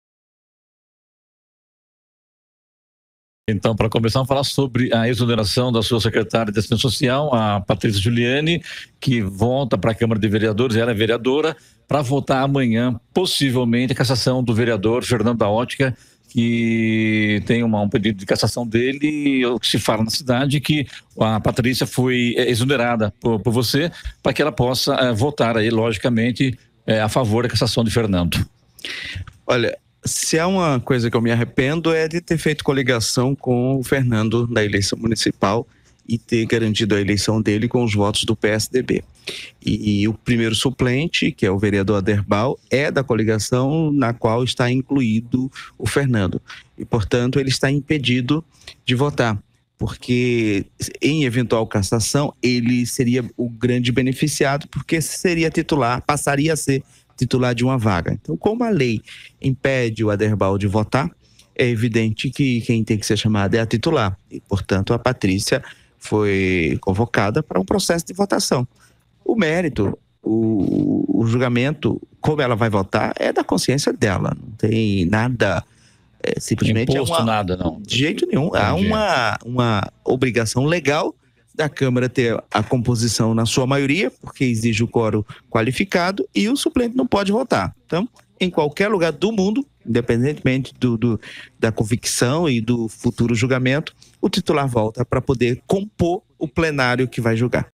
Prefeito Izaias fala sobre a cassação do vereador Fernando da Ótica
Prefeito de Jacareí, Izaias Santana, fala sobre a cassação do vereador Fernando da Ótica e a volta da secretária Patrícia Juliane a cadeira de vereadora para votar.